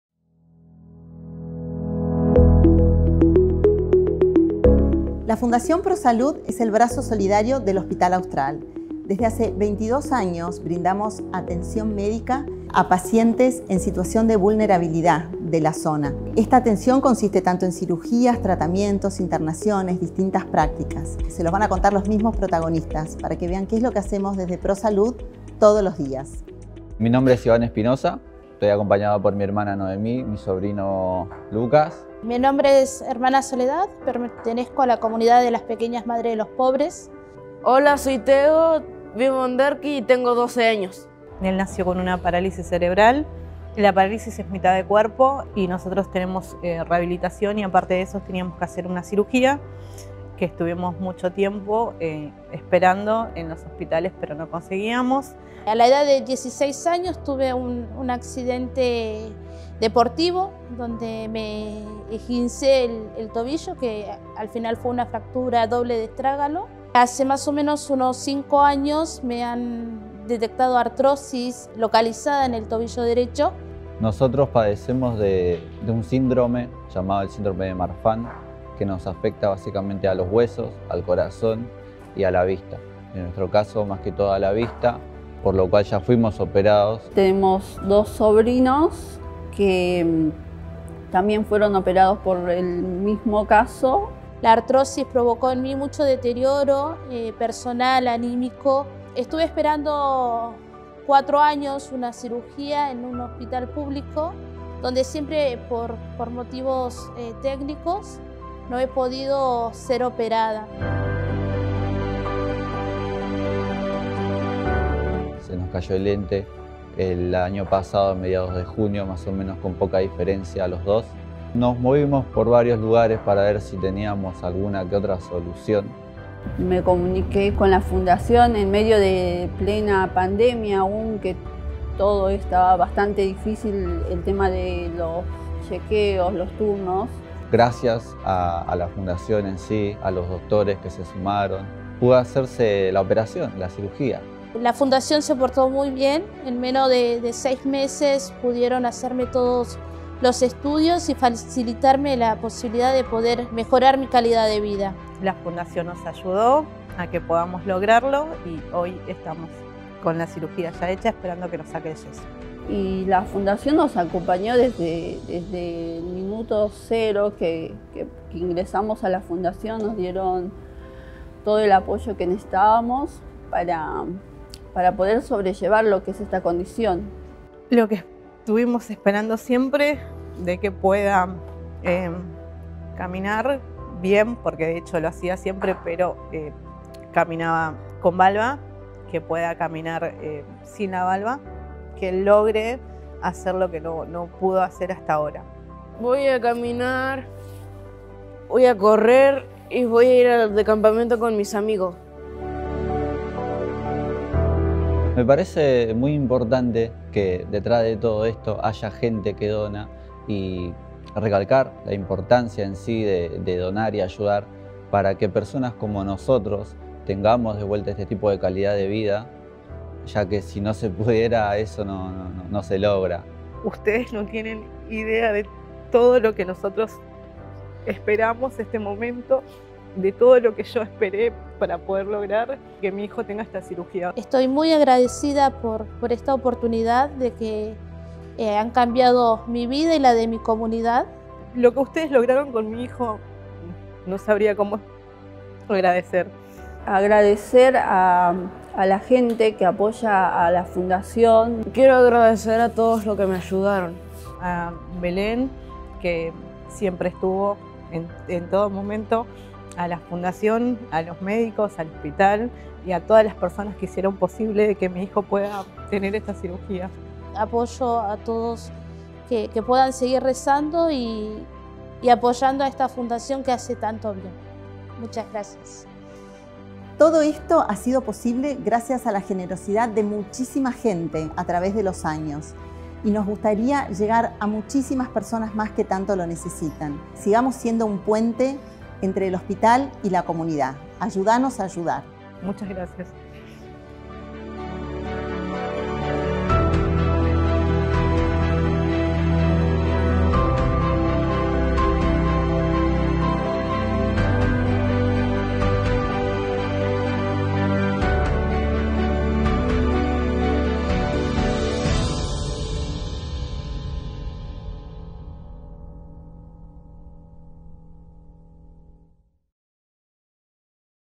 Fundación ProSalud
Desde los inicios del Hospital, nuestra Fundación ProSalud llevó a cabo más de 450 cirugías y 15.000 prestaciones ambulatorias y estudios de complejidad.